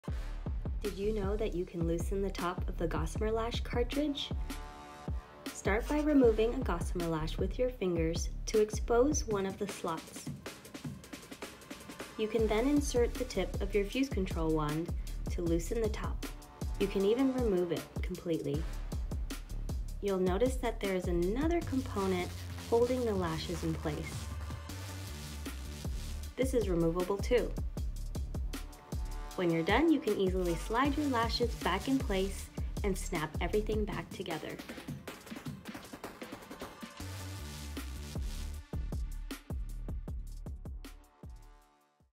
How to Loosen the Cartridge
How to loosen the Gossamer® lash cartridge for easier removal of our lashes prior to application.